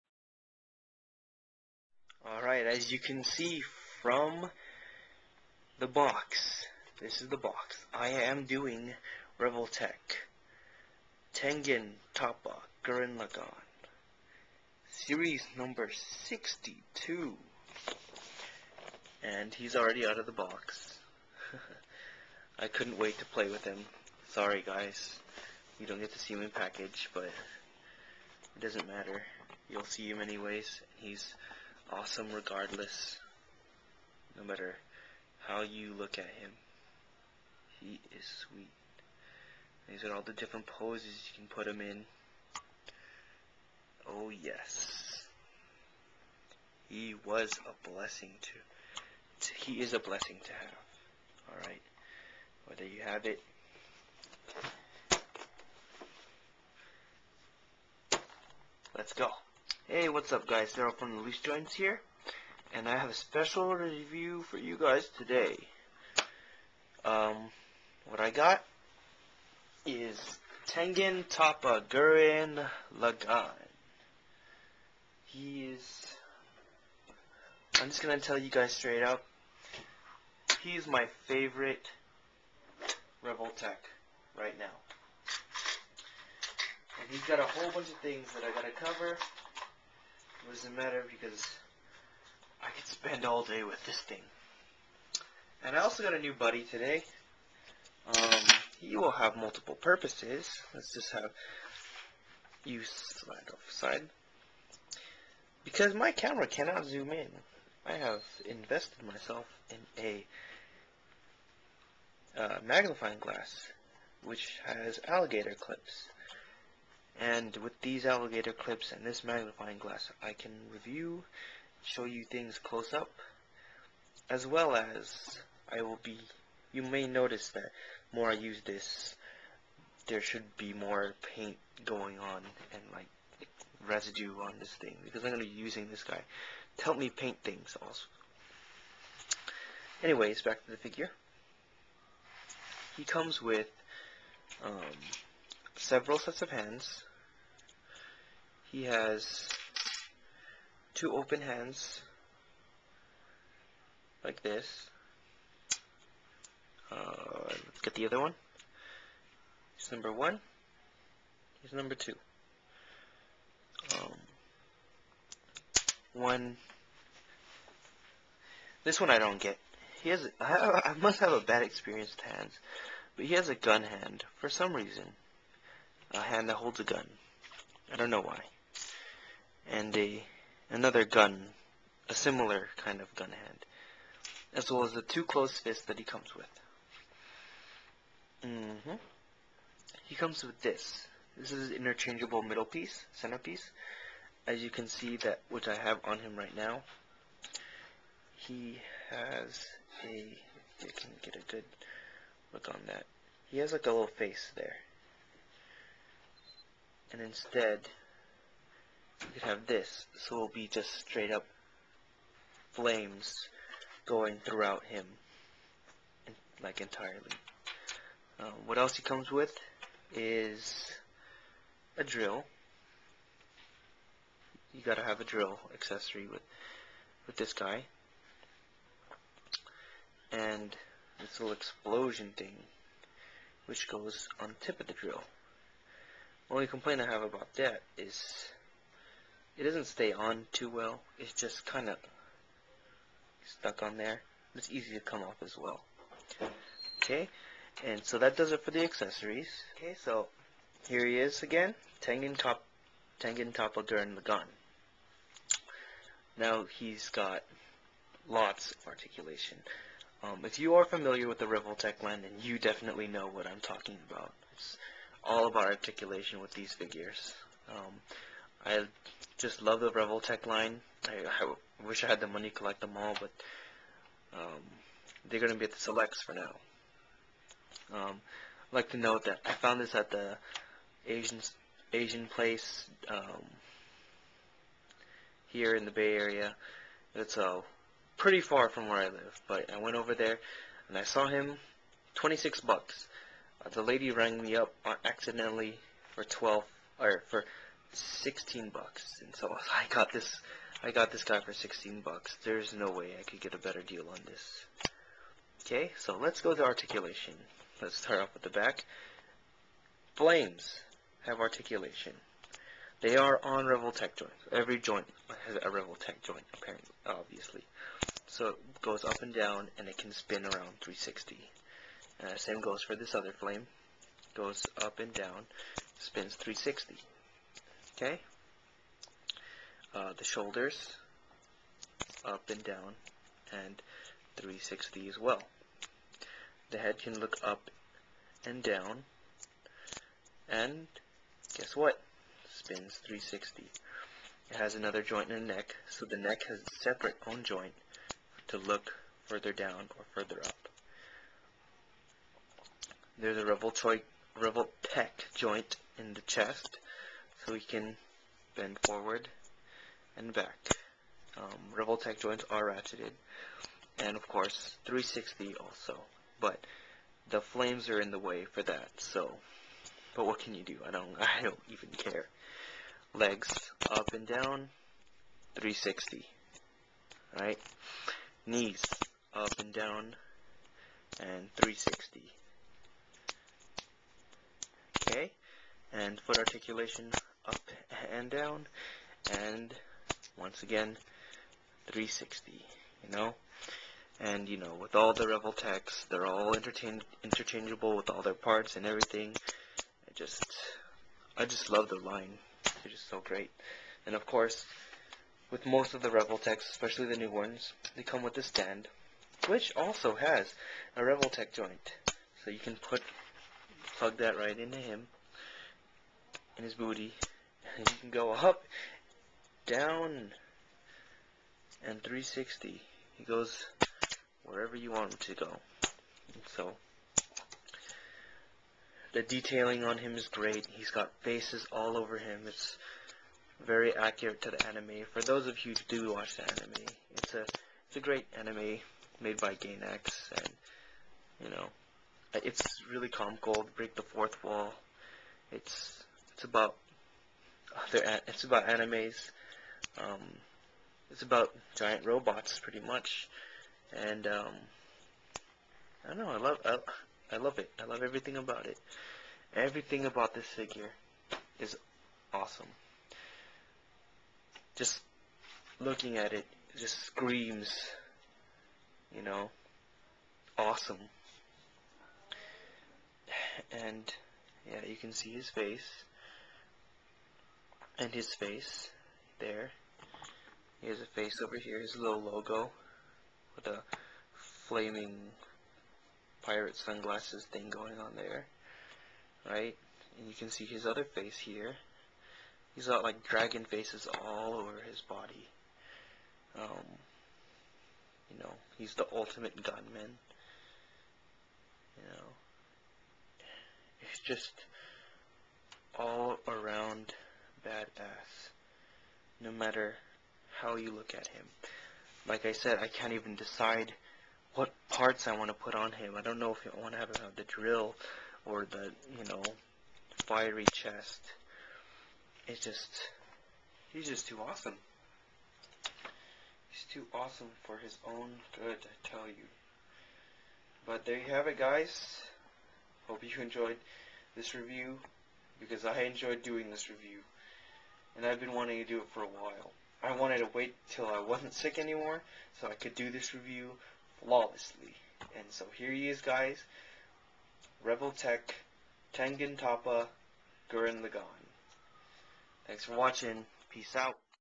TLJ: Revoltech Tengen Toppa
Revoltech Tengen Toppa Gurren Lagann review.  My favorite Revoltech!